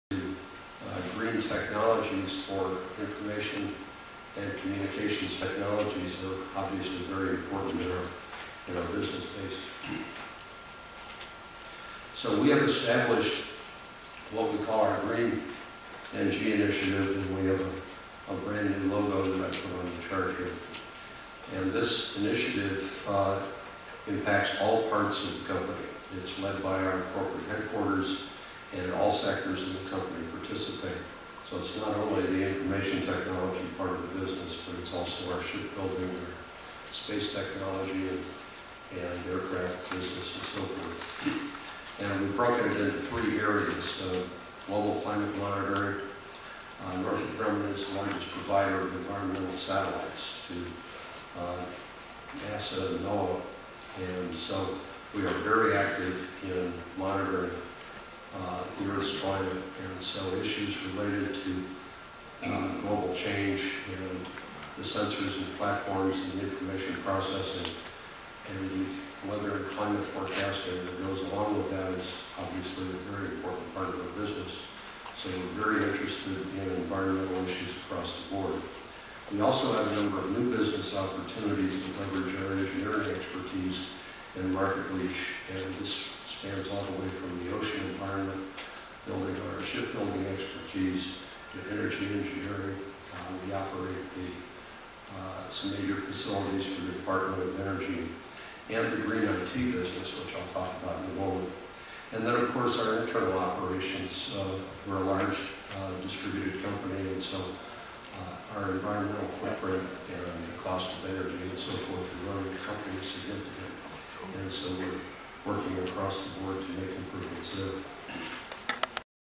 Northrop Grumman's Green NG Intiative
Senior Vice President and Chief Technology Officer of Northrop Grumman Information Technology, Dr. Robert F. Brammer discusses the importance of green technology issues to Northrop Grumman.  Brammer details Northrop's introduction of their "Green NG" initiative which is lead by corporate headquarters and effects all parts of the company.